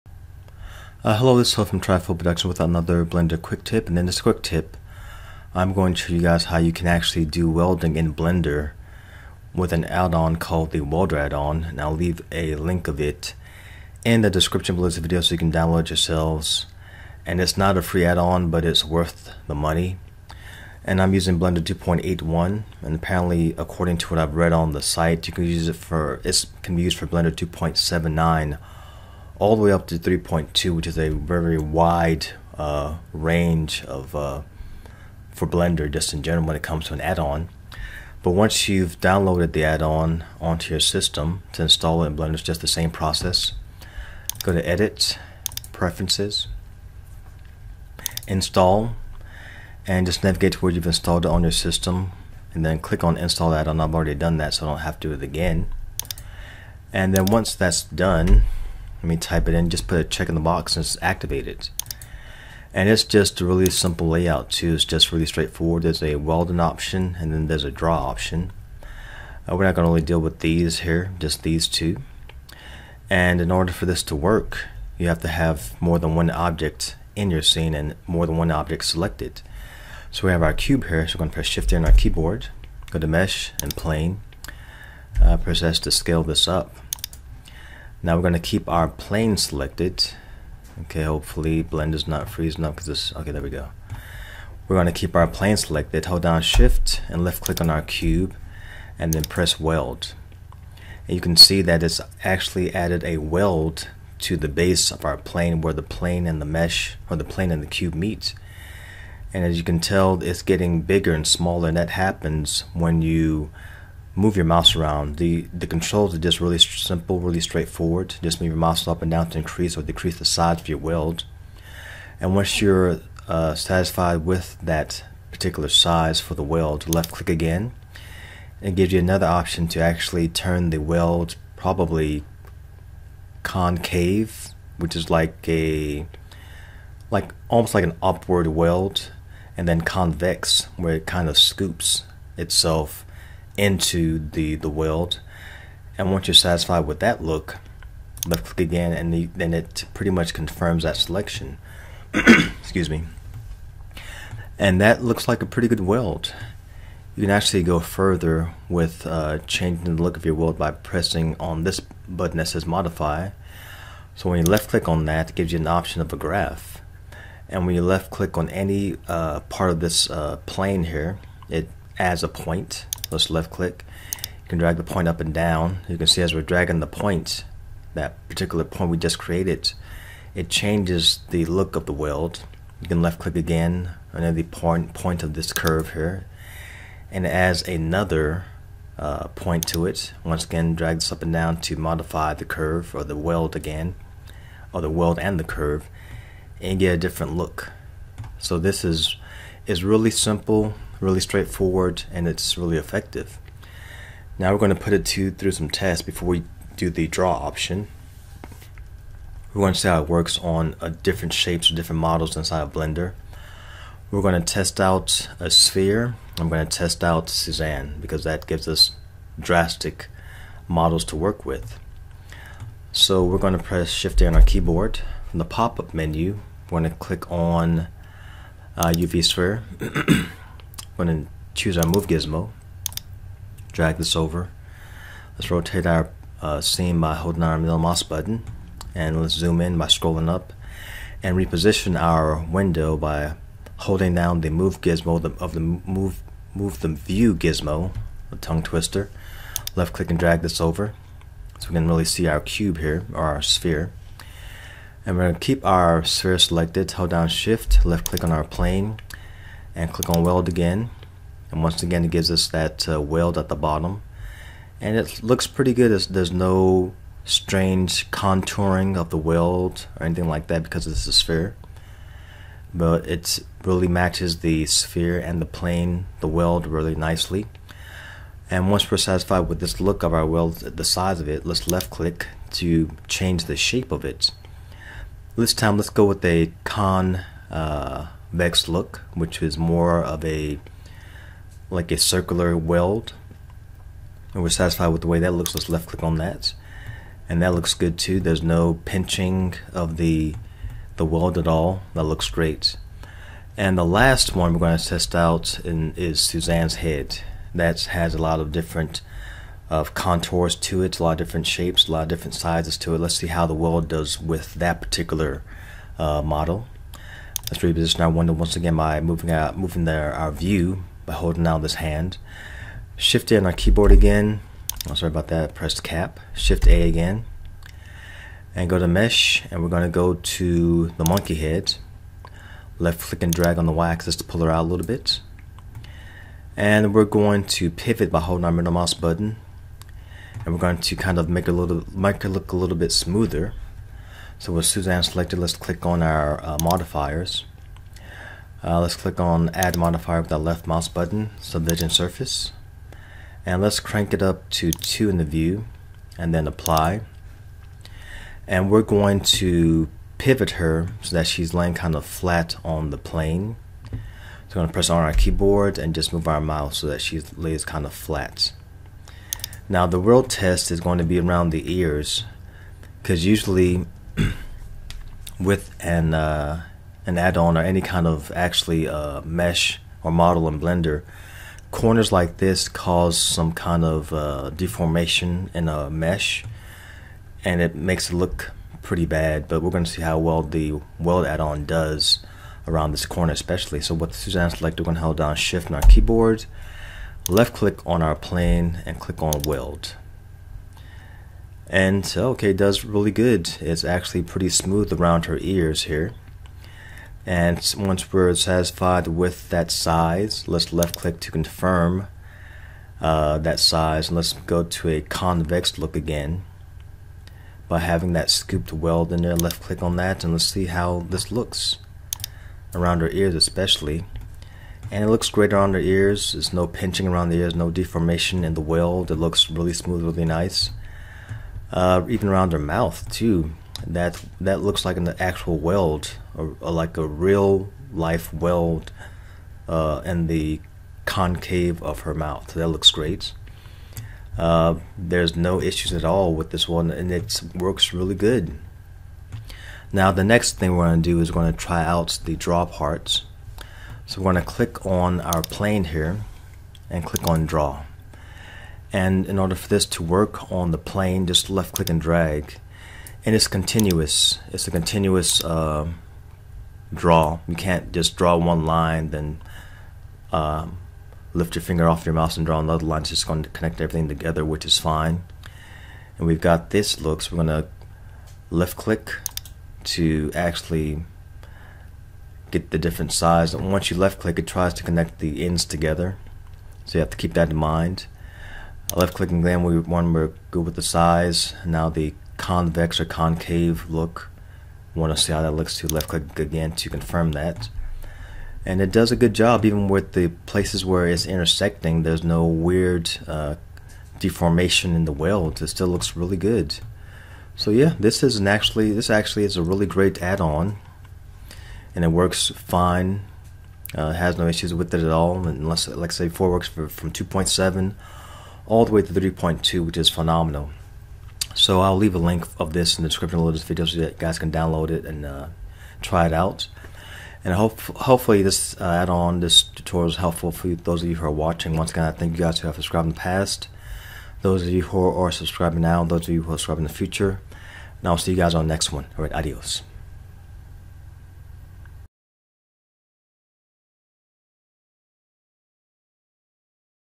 Blender Quick Tip: Welder addon
In this tutorial, you'll learn how to easily apply a 'weld' mesh onto any model using this addon.